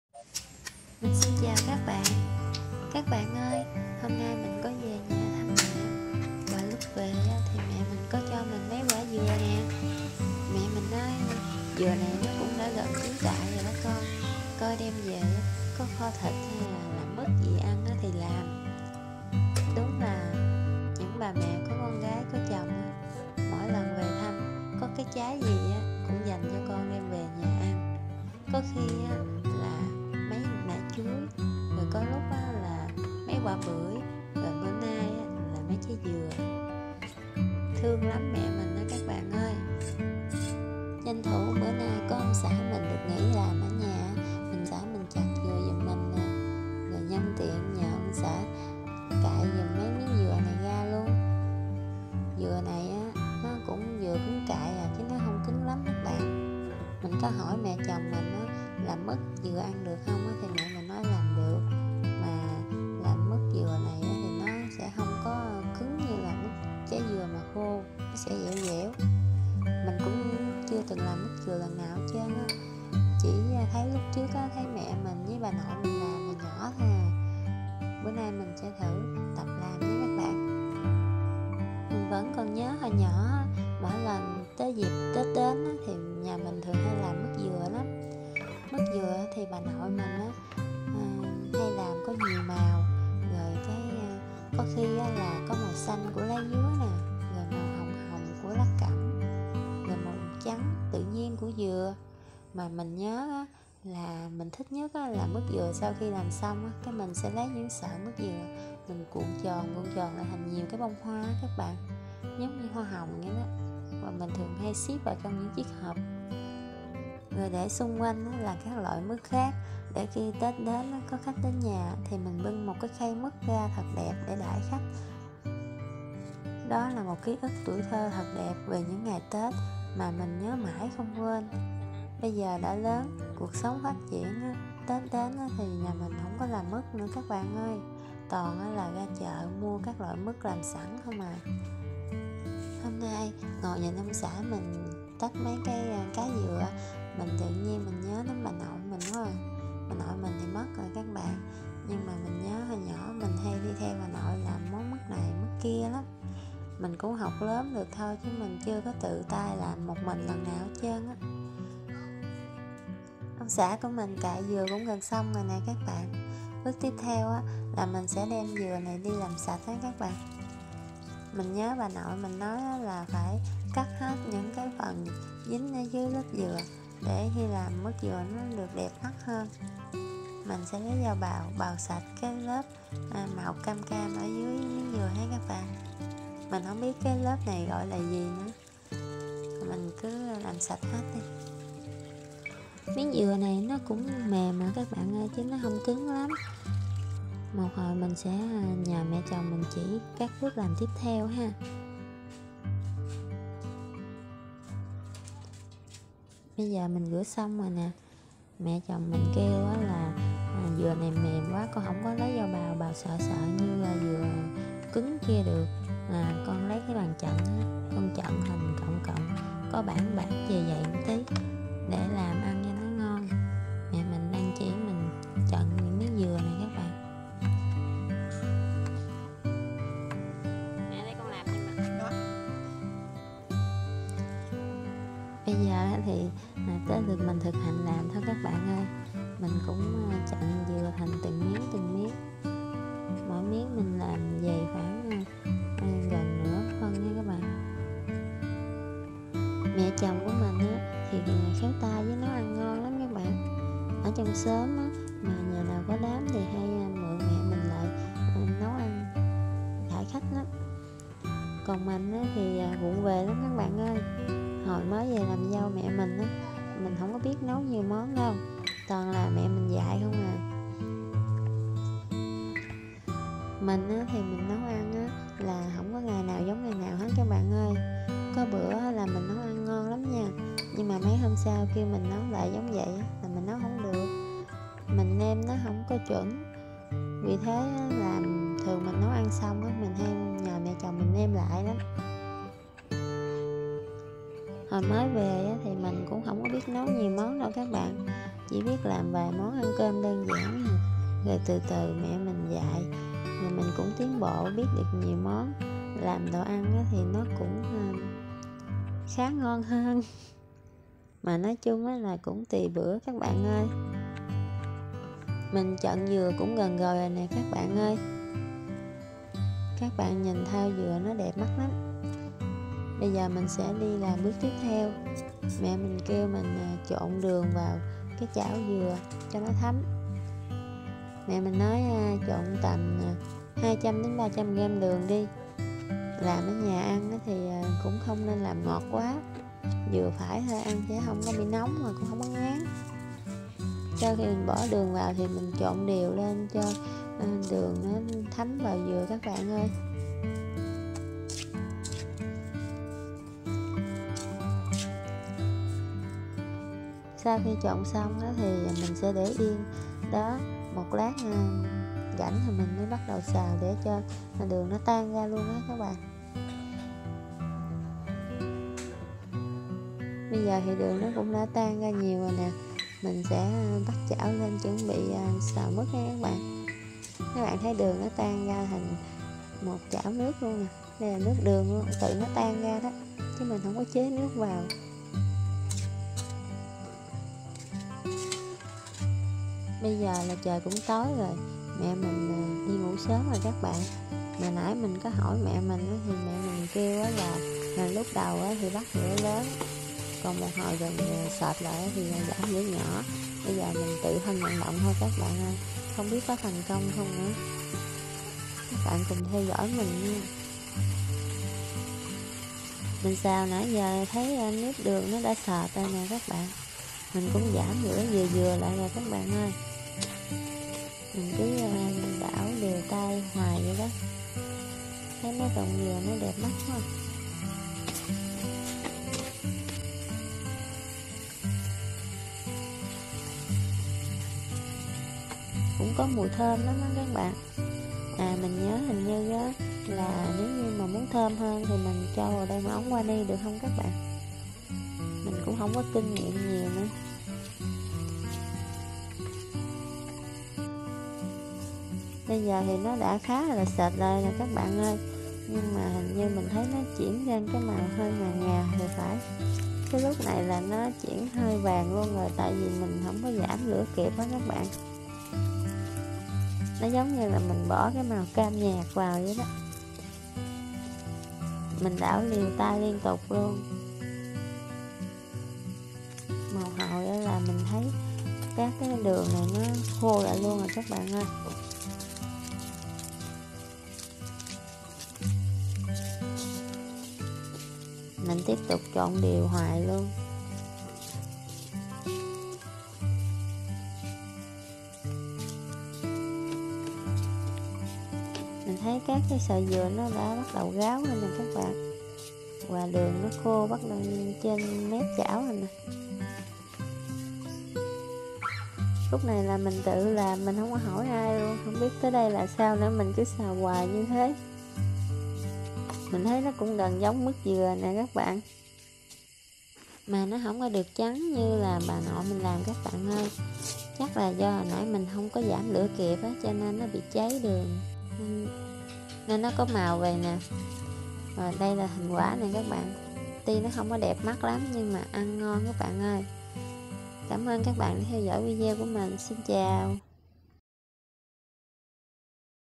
Lần đầu tập làm mức dừa và cái kết... | Ẩm thực miền Tây| Diểm Phi Vlog
Mẹ cho mấy trái dừa cứng cại, mình tập làm mức dừa và cái kết bất ngờ.
Chúc cả nhà xem video vui vẻ nha...